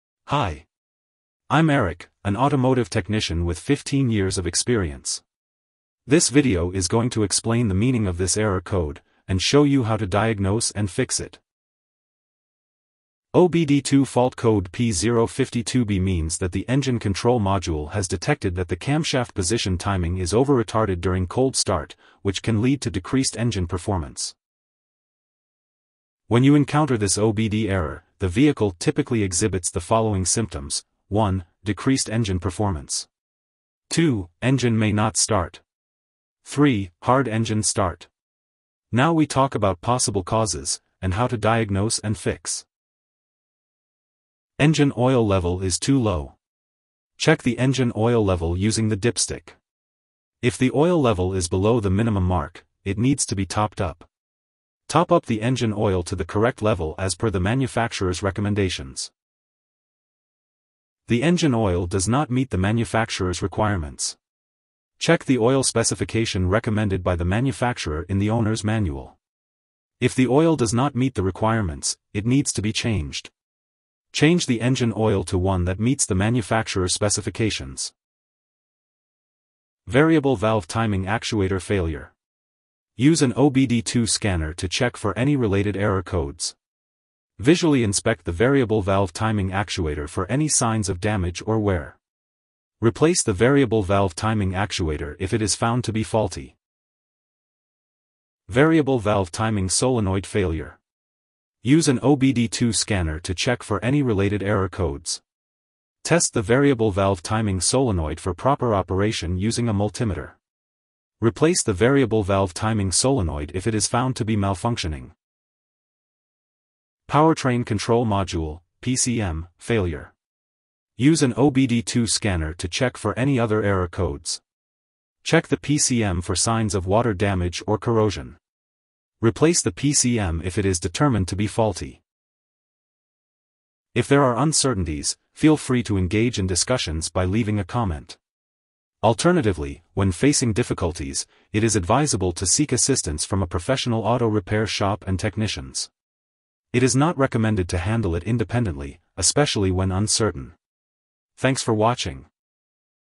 How to Diagnose and Fix P052B Engine Code - OBD II Trouble Code Explain
OBD-II fault code P052B means that the engine control module has detected that the camshaft position timing is over-retarded during cold start, which can lead to decreased engine performance.
4b70